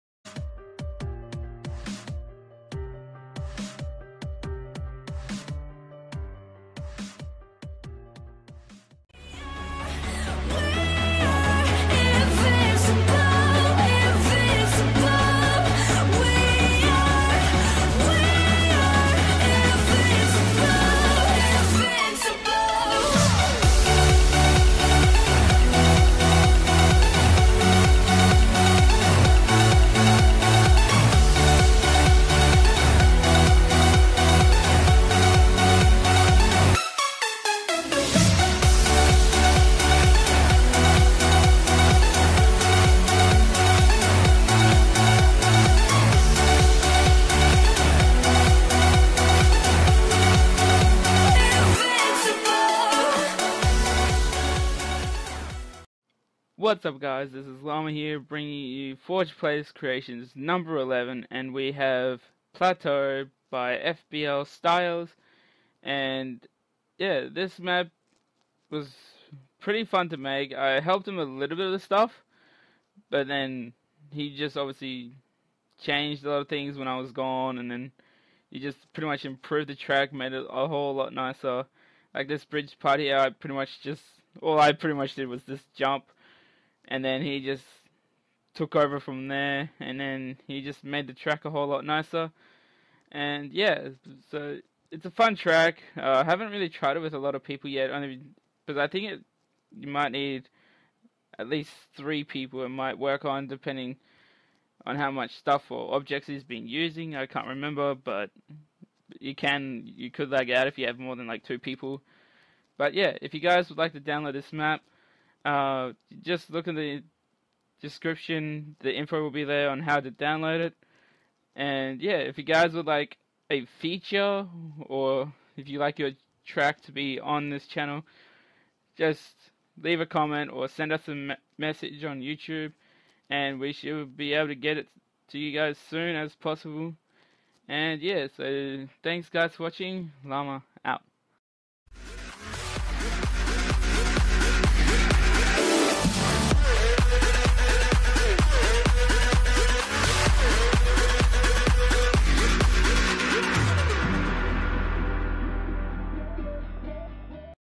ForgePlaylist Creations #11 Plateau MT: by FPL Styles
FPL Styles with his newest track, Plateau MT. featuring GP, Terrain and mx